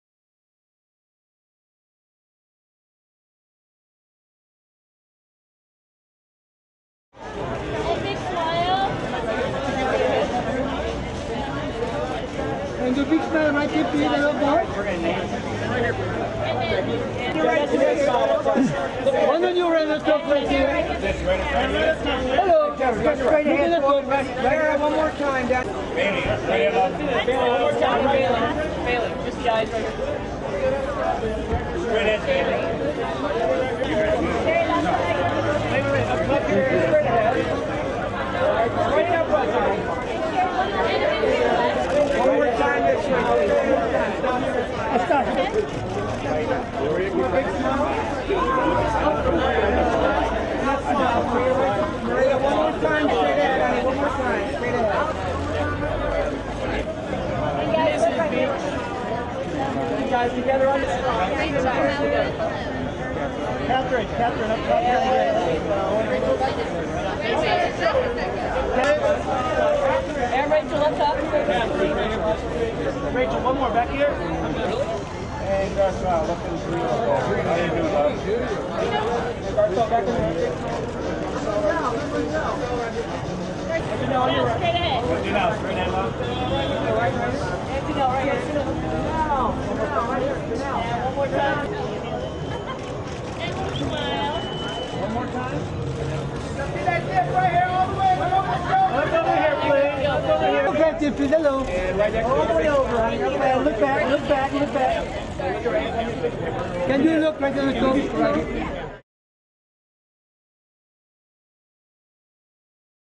Cara Santana & Jesse Metcalfe all smiles...
Stars walked the red carpet at the People Stylewatch Denim Party in Hollywood.